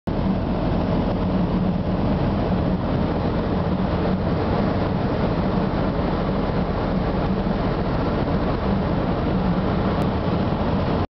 Subaru Vanagon 2.5 Caravelle "S"
Following Dad's DOHC 2.5 Subaru powered Vanagon Caravelle S.